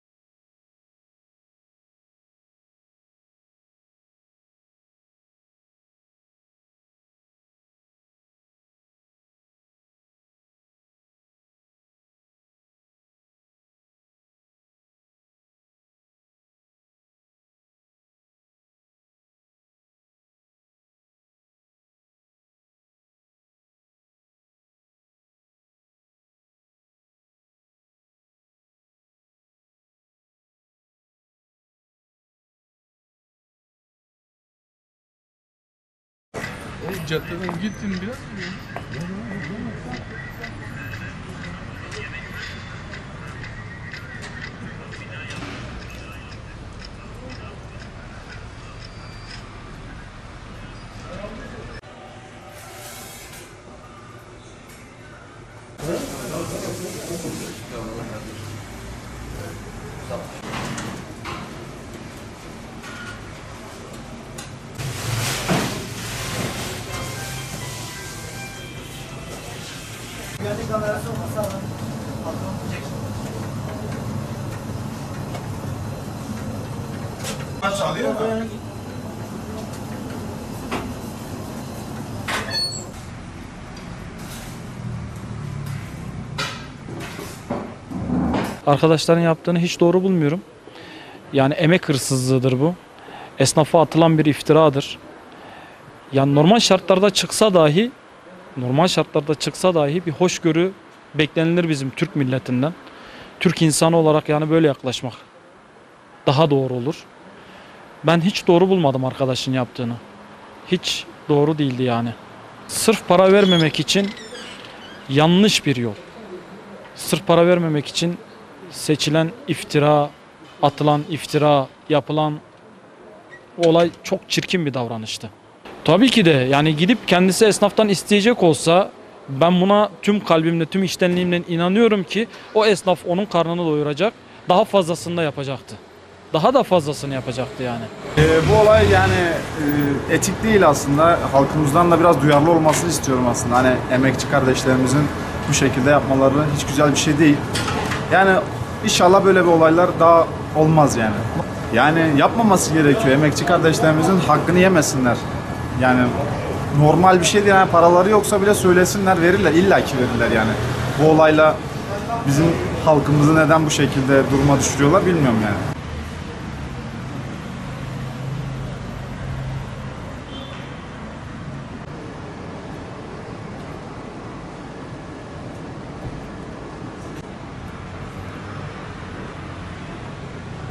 Para ödememek uğruna bunu da yaptı! Saçını koparıp yemeğe koydu
Elazığ'da bir kişi, yediği yemeğin parasını ödememek için saçından kopardığı kılları, yemeğin içine koydu. O anlar iş yerinin güvenlik kamerasına yansırken, yaşanan olay, Kemal Sunal’ın rol aldığı Meraklı Köfteci filmindeki sahneyi akıllara getirdi.

TURKSAT A4
D-SMART GO
DIGITURK: 51